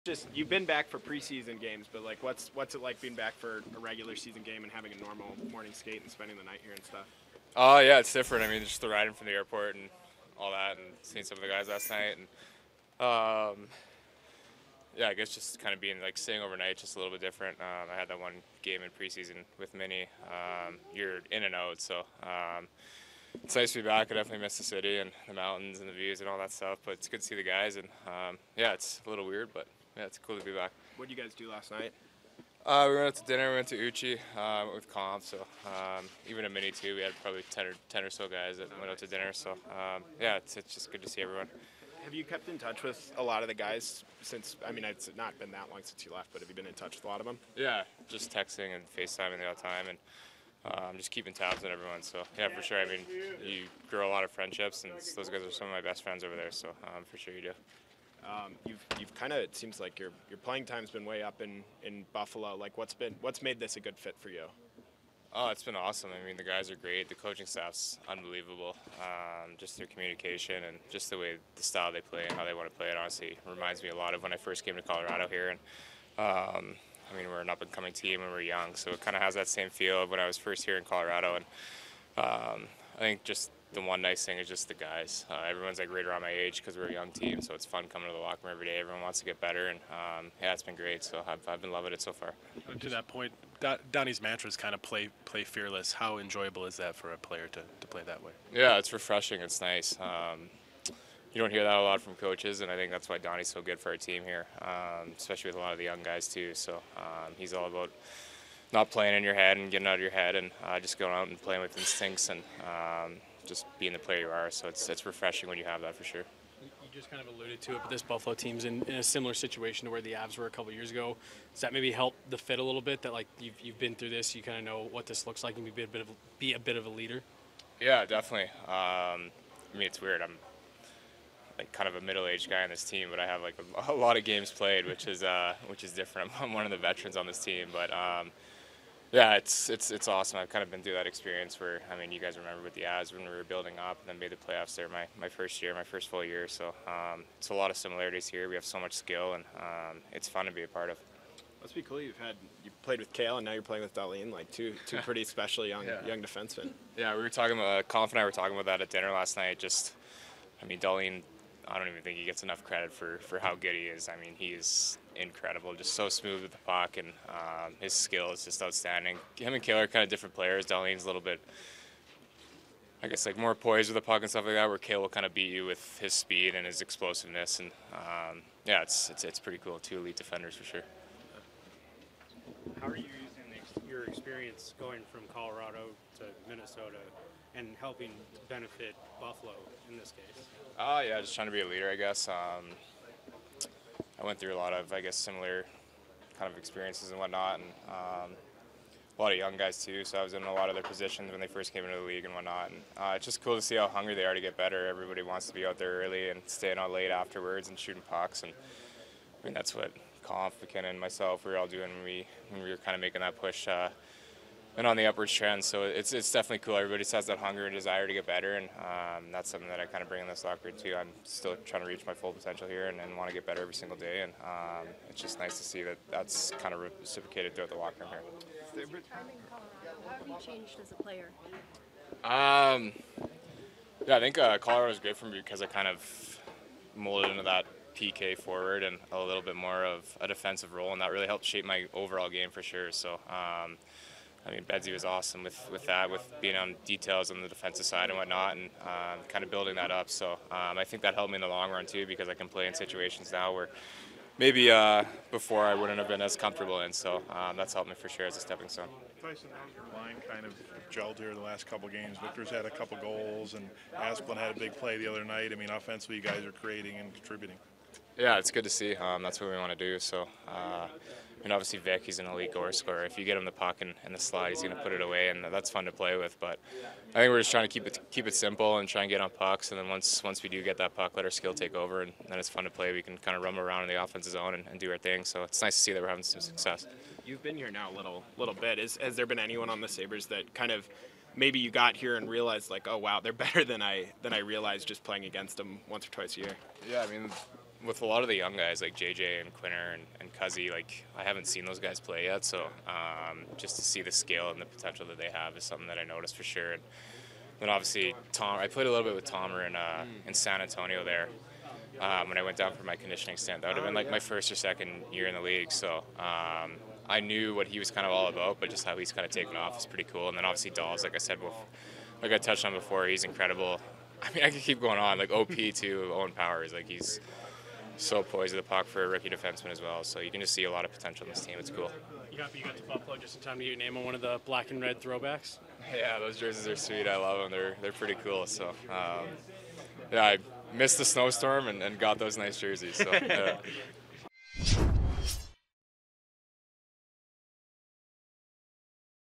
"Hungry To Get Better" | Tyson Jost Before Game Against Colorado Avalanche
Buffalo Sabres forward Tyson Jost addressed the media on December 15th, 2022 before the team's game against the Colorado Avalanche. Topics include: his return to Colorado, time in Buffalo so far, the Sabres' potential, comparing Makar to Dahlin, what makes Don Granato a great coach, and more.